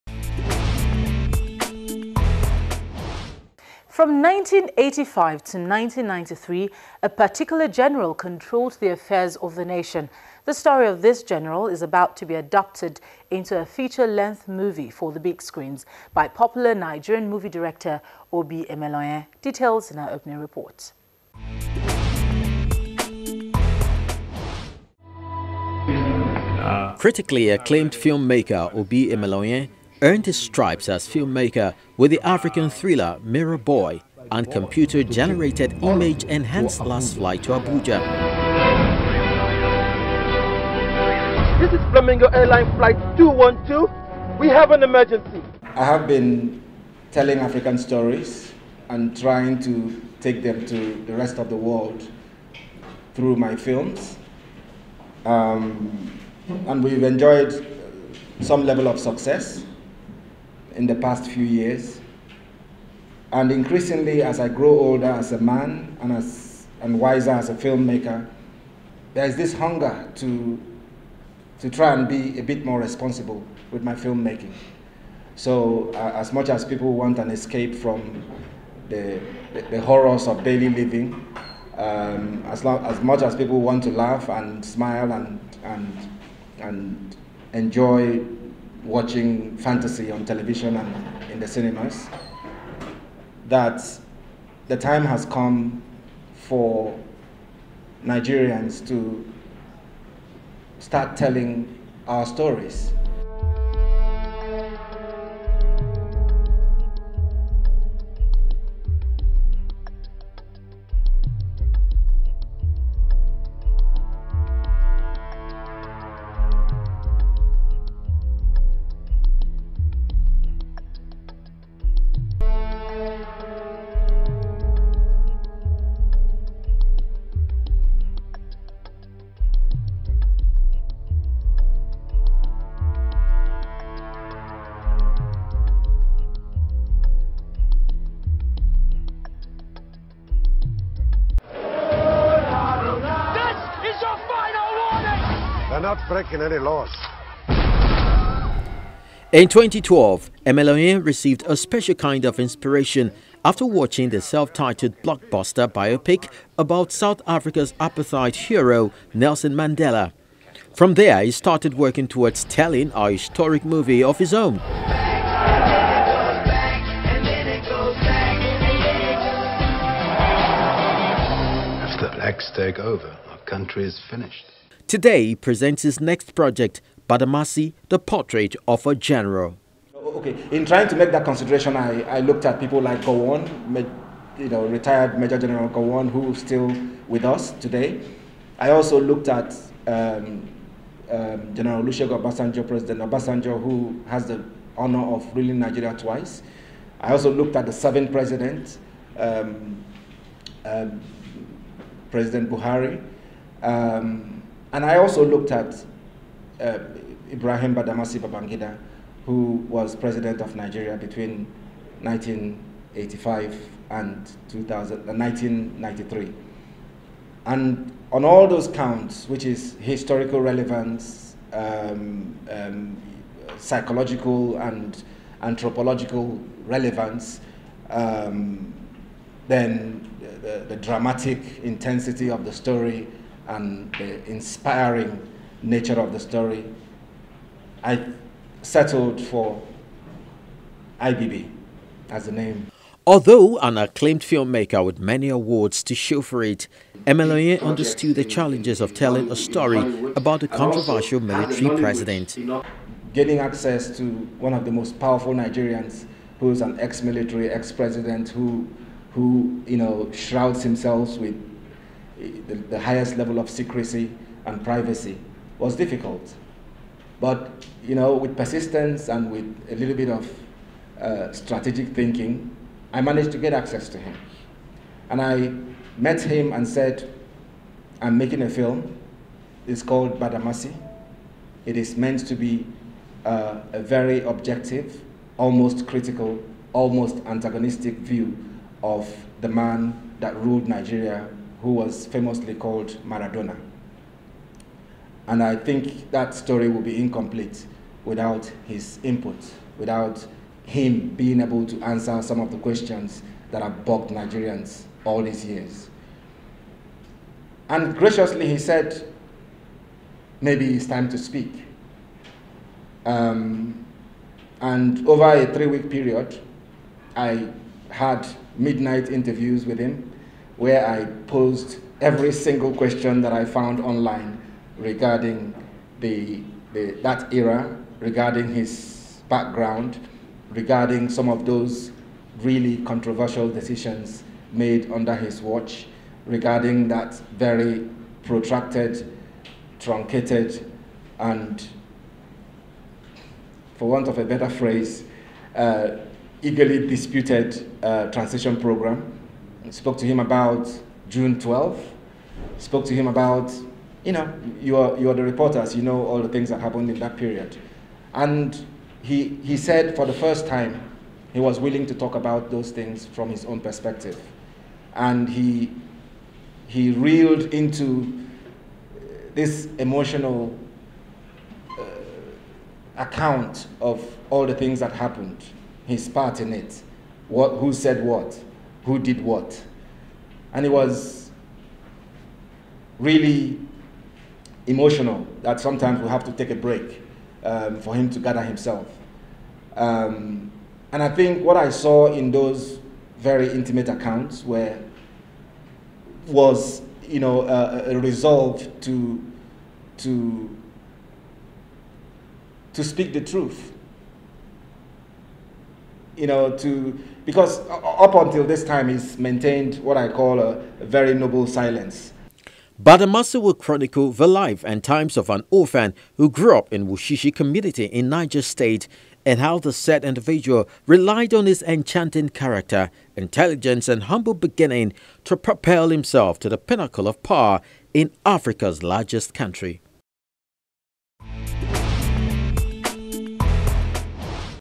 EN: Obi Emelonye To Create Movie About Military General, IBB
Nigerian movie maker, Obi Emelonye set to adopt the story of Military General Ibrahim Babangida into a movie.